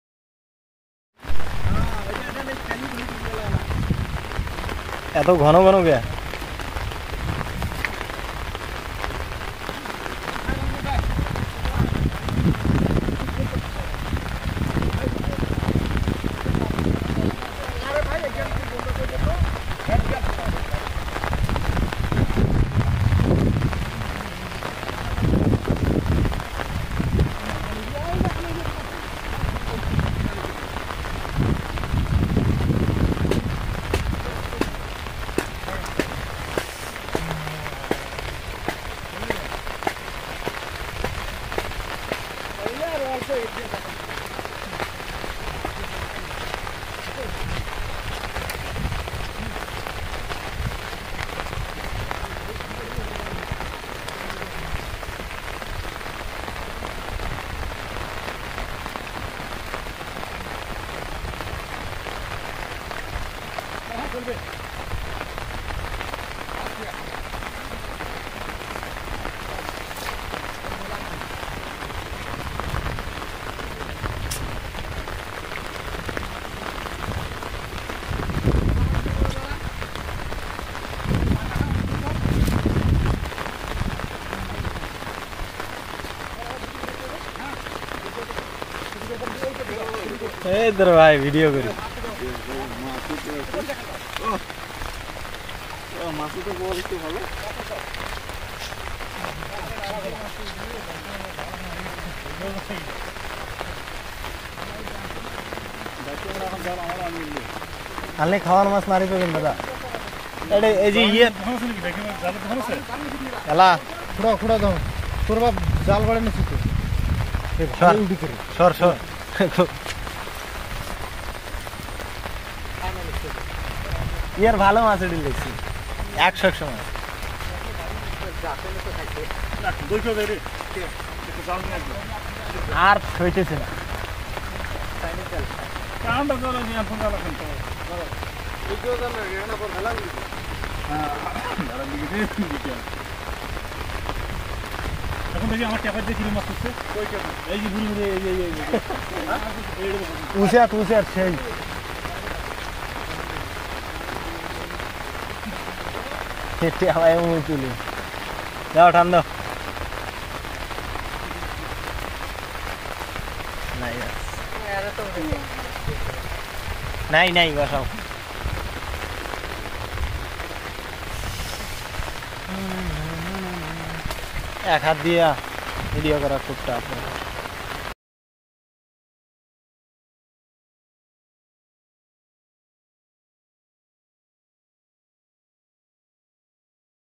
Traditional Flood Water Fishing | Smart Boy Catching Country Fish in Heavy Rain
In this video you will watch amazing fishing in village .